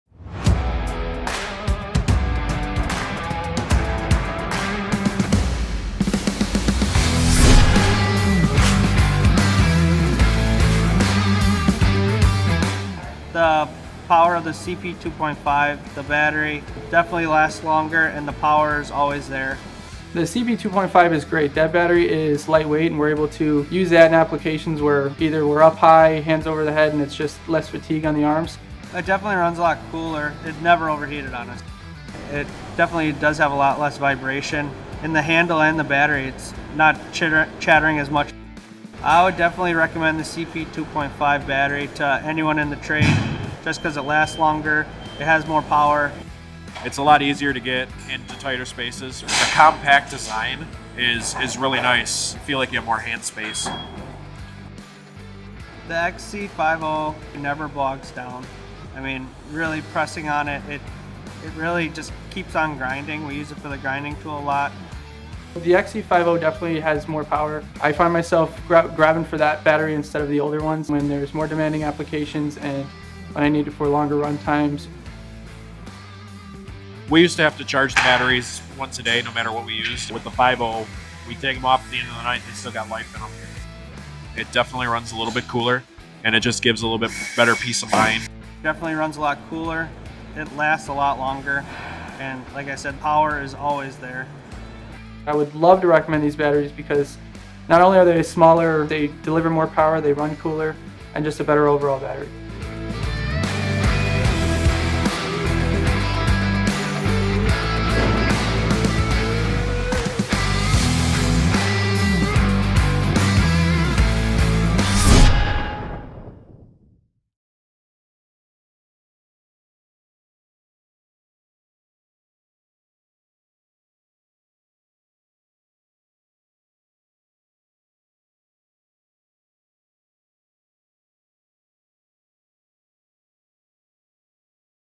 Milwaukee® M12™ REDLITHIUM™ HIGH OUTPUT™ Batteries
Our M12 REDLITHIUM™ HIGH OUTPUT™ CP2.5 Battery Pack delivers 25% more power and runs 25% cooler than our other M12 REDLITHIUM™ CP battery packs. This increased performance of the lithium-ion battery gives you the same power and runtime of an M12 REDLITHIUM™ XC battery in a lighter and more compact package. The 12-volt battery pack delivers fade-free power and runs substantially cooler through demanding applications, allowing you to push your cordless power tools throughout a wide range of applications. Its compact, lightweight design allows you to work in tight workspaces and reduces fatigue. Each battery pack is durably built for heavy-duty use, runs cooler, and performs in climates below 0°F/-18°C. In addition, the M12 REDLITHIUM™ HIGH OUTPUT™ CP2.5 is built with a resistant housing, providing increased protection against exposure to common oils, greases, and solvents found in automotive and manufacturing environments. Our M12 REDLITHIUM™ HIGH OUTPUT™ CP2.5 is fully compatible with 125+ M12™ solutions.